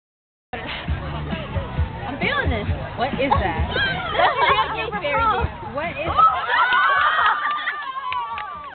The Gay Jiggle
Sean & Bowman so gracefully present the gay dance in perfect unison at Relay for Life, helping to cure cancer.